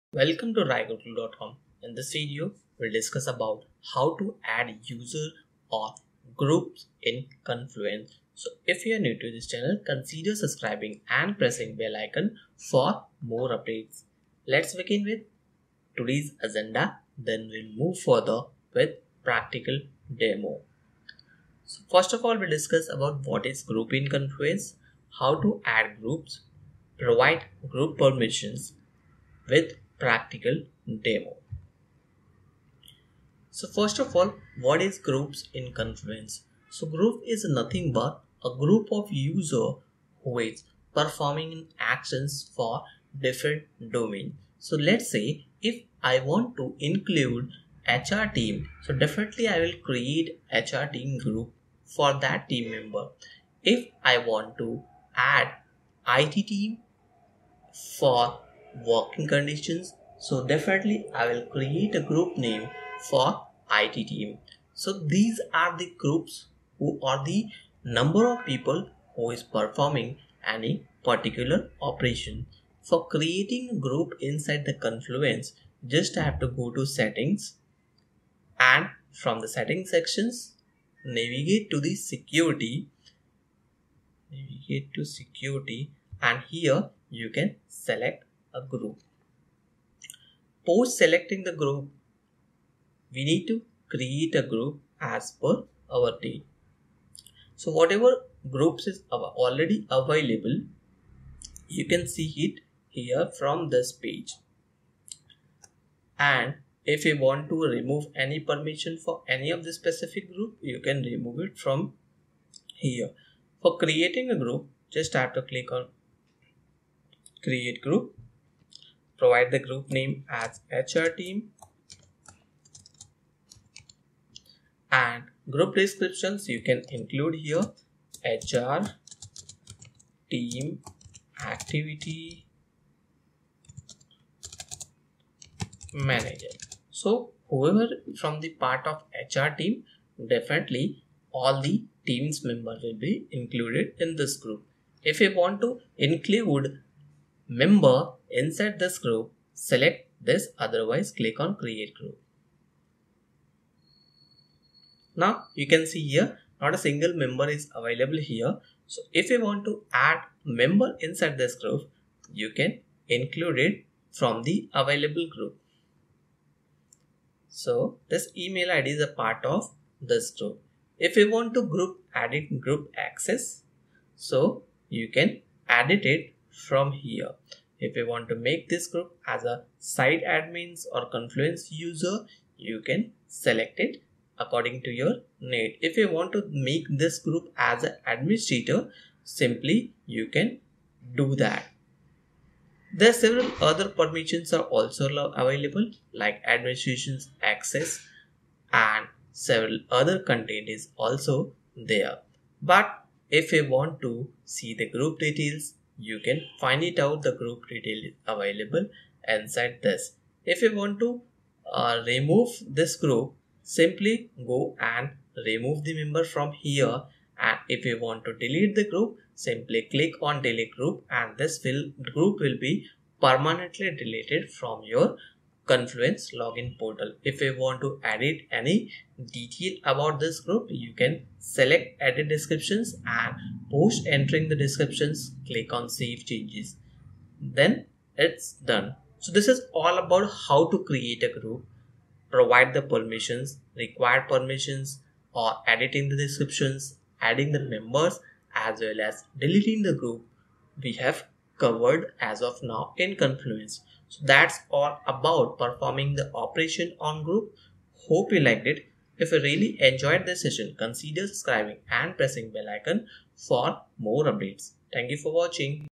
How to Add and Manage Group in Confluence | Confluence Tutorial
Grouping users in Confluence is a great way to cut down the work required when managing permissions and restrictions.

If you're a space admin, you can assign a set of space permissions to a group rather than to each individual user. And as a page creator with 'Add/Delete Restrictions' permission, you can also add and remove page restrictions for groups.

Default Confluence groups
There are two default groups in every Confluence instance and, beyond that, Confluence administrators are free to set up and edit groups in any way they see fit.

The two default groups in Confluence are:

confluence-users - this is the default group into which all new users are usually assigned. In most sites this is the group that provides the permission to log in to Confluence.
confluence-administrators – this super group grants the highest level of administrator permissions. Members of this can view all pages, including restricted pages. While they can't edit existing pages, they can add, delete, comment, restore page history, and administer the space. They can also access the admin console and perform all administrative tasks.